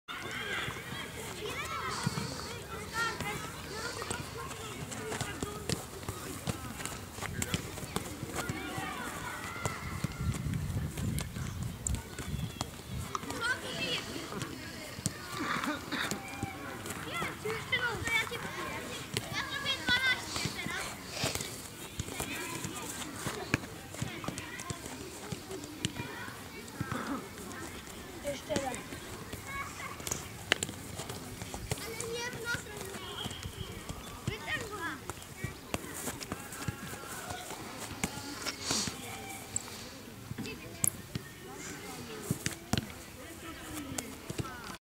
Trening piłkarski Górnika Libiąż - cz.30
Ćwiczenie zwodu polegającego na przeciągnięciu piłki podeszwą w tył i zagraniu jej wewnętrzną cześcią stopy do boku.

Wskazówki:
- najpierw opanujmy przeciągnięcie piłki w tył, a dopiero później dodajmy drugą część ćwiczenia